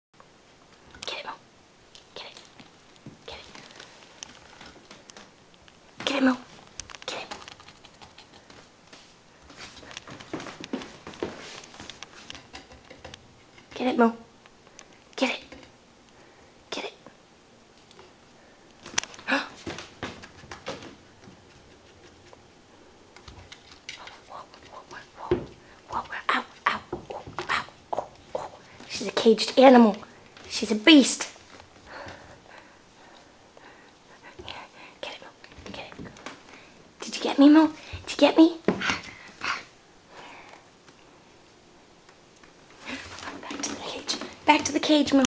Moo fights my hand
Starts off slow, but look at her funny back legs and how she is perched on the stool!
SO FUNNY!